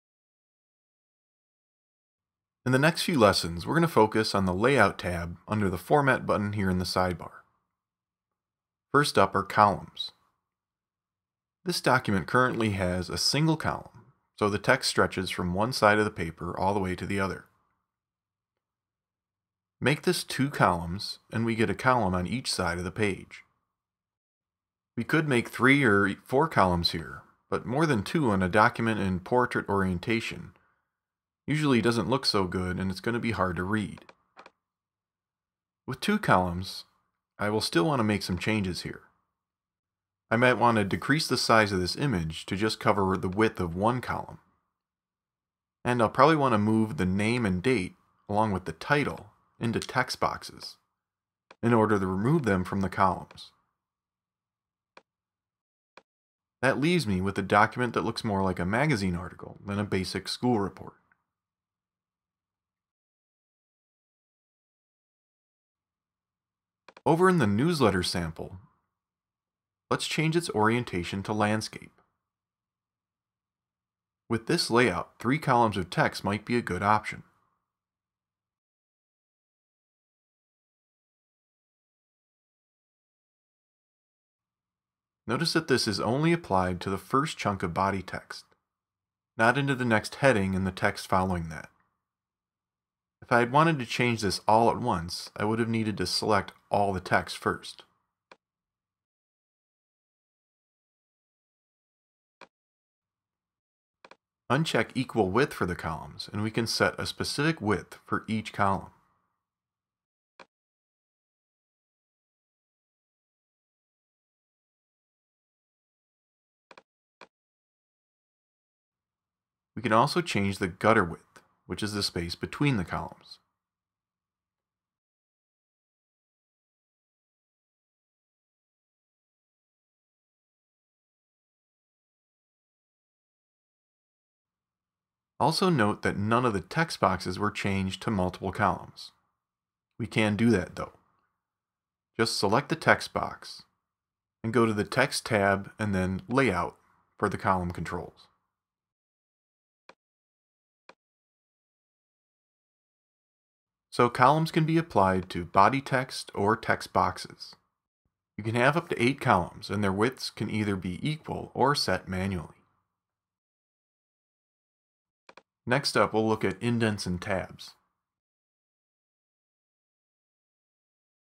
How to Layout Text in Columns with Pages for Mac.
In this lesson from our full Pages for Mac Tutorial see how to layout your text in columns either within a text box or on one or multiple pages.